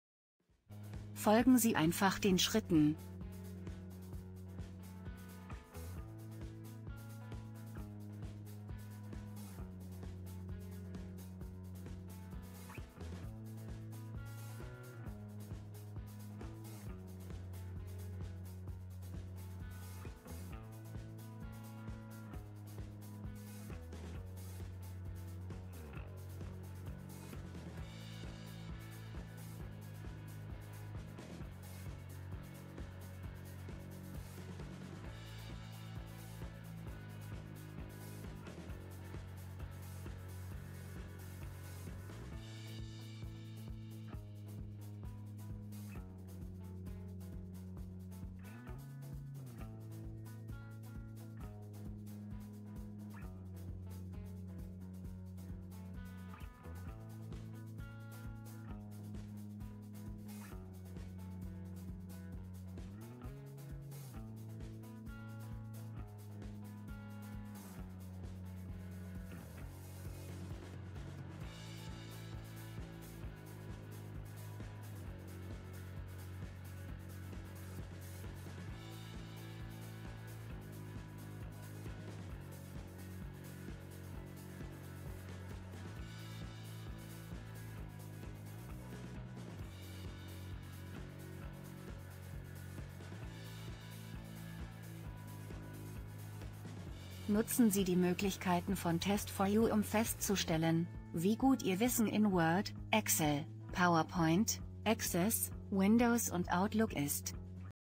Δndern Sie die Abfrage Bestellung, so dass sie nur die Felder ProduktID, Beschreibung, Preis und...
Δndern Sie die Abfrage Bestellung, so dass sie nur die Felder ProduktID, Beschreibung, Preis und KundenAnzahl für jeden Produkt enthält und anzeigt. Das Feld KundenAnzahl sollte ein berechnetes Feld sein, das die Kunden zählt, die Produkte in einer Menge zwischen 10 und 20 Artikel gekauft haben.
Die Felder sollten in der oben angegebenen Reihenfolge angezeigt werden.
Um die Kunden zu zählen verwenden sie das Feld KundenID.
Speichern und schließen Sie die Abfrage.Möchten Sie mehr. Laden Sie das TEST4U demo herunter